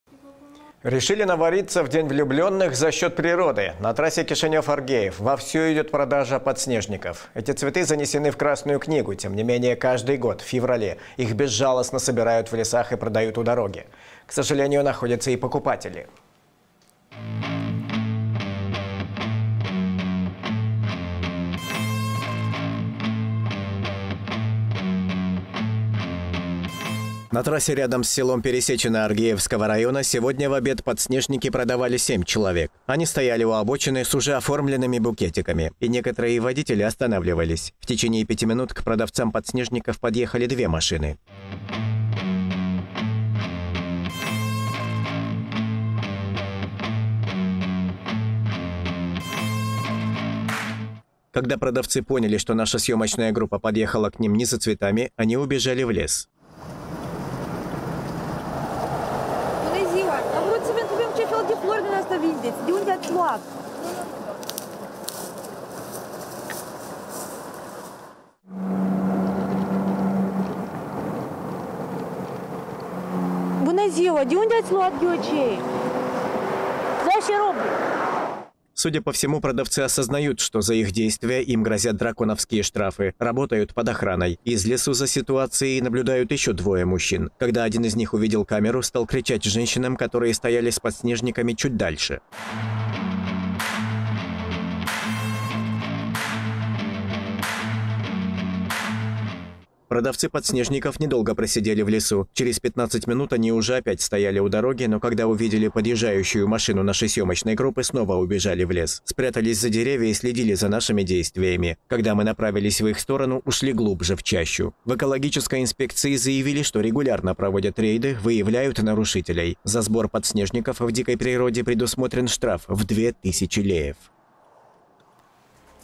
На трассе вовсю идет торговля подснежниками
Решили "навариться" в День влюбленных за счёт природы: на трассе Кишинёв-Оргеев вовсю идёт продажа подснежников. Эти цветы занесены в Красную книгу, тем не менее, каждый год в феврале их безжалостно собирают в лесах и продают у дороги. К сожалению, находятся и покупатели.